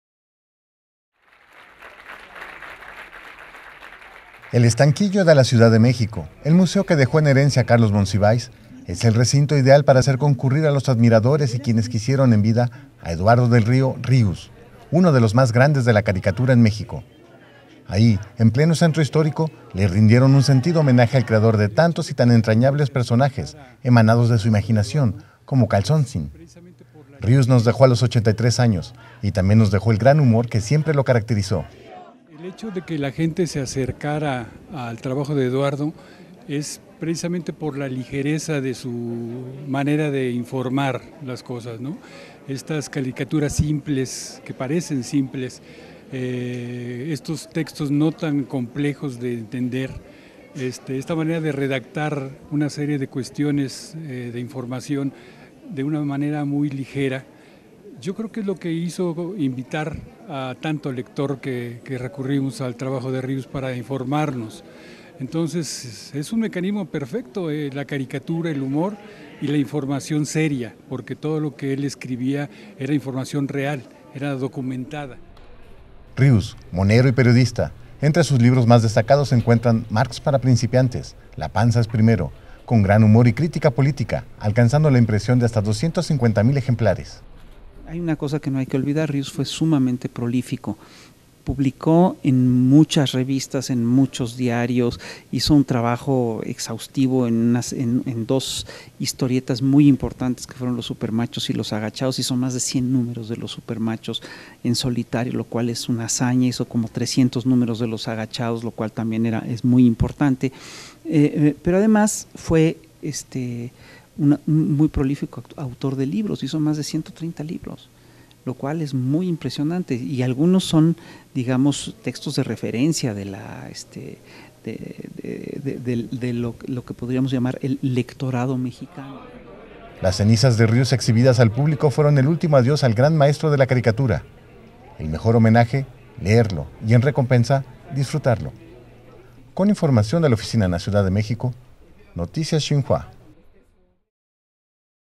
Adiós a Rius; lo despiden caricaturistas y familiares en el Estanquillo
El Estanquillo de la Ciudad de México, el museo que dejó en herencia Carlos Monsiváis, es el recinto ideal para concurrir los admiradores y quienes quisieron en vida a Eduardo del Río, Rius, uno de los más grandes de la caricatura en México.